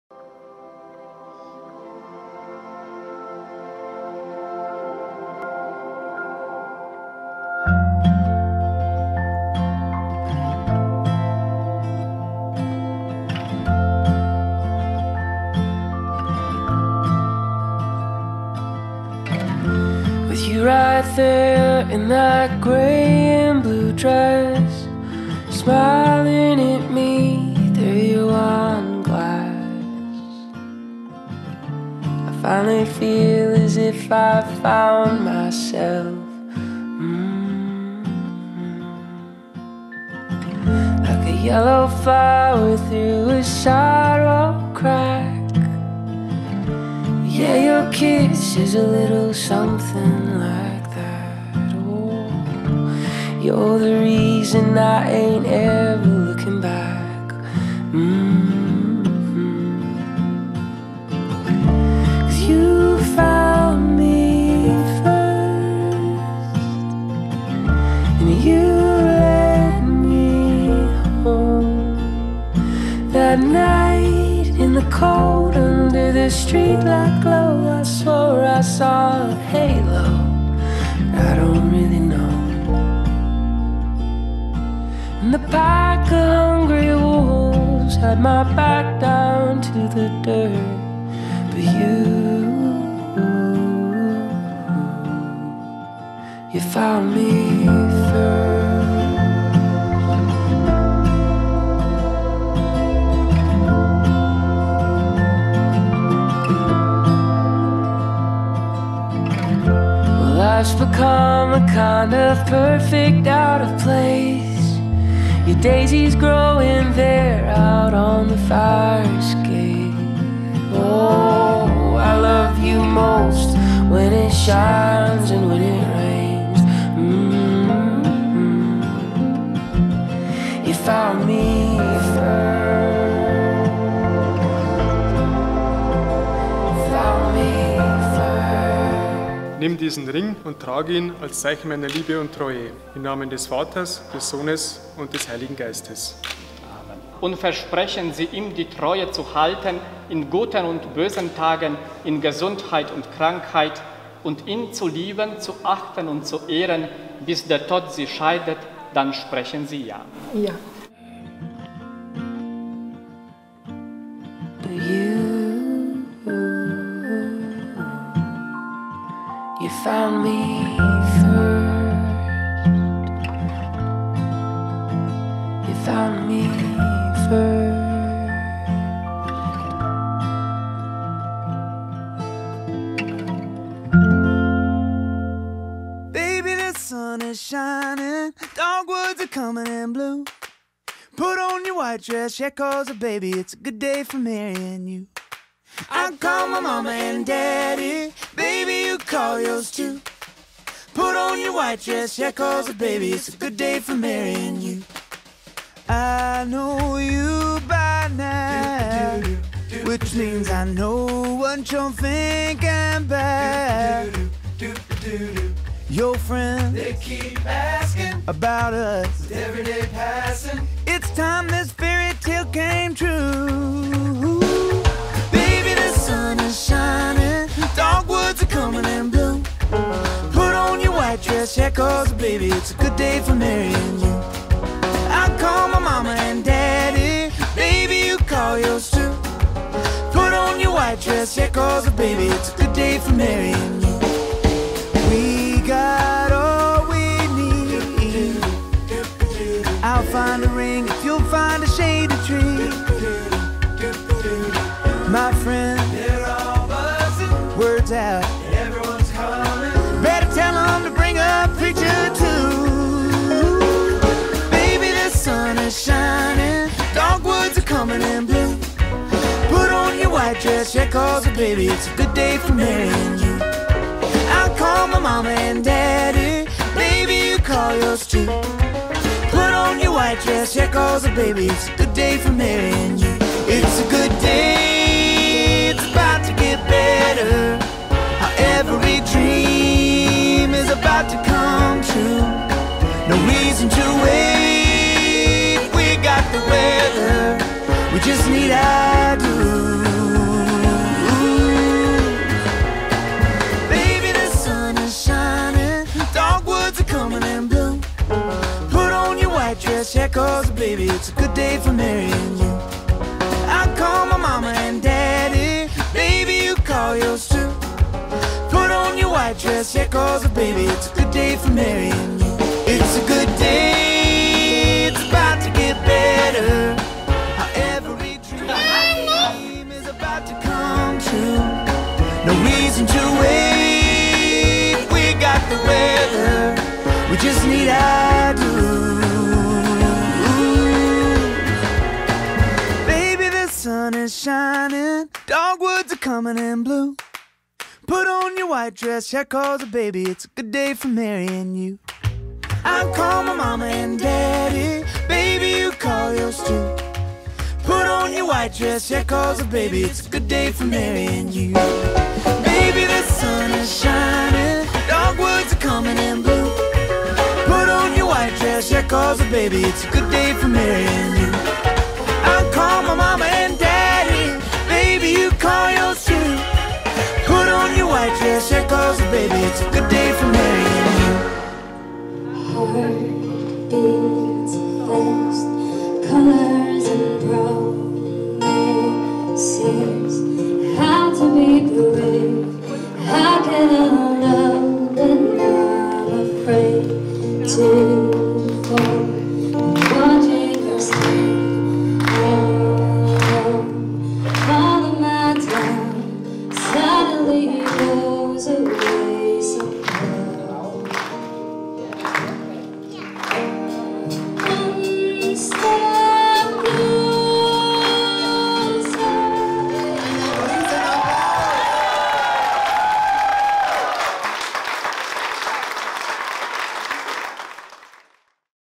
Hochzeitsfilm Angela & Gerhard Hochzeitsmoment
In diesem Hochzeitsvideo zeigen wir Euch romantische und bewegende Momentaufnahmen von Angela & Gerhards Hochzeit.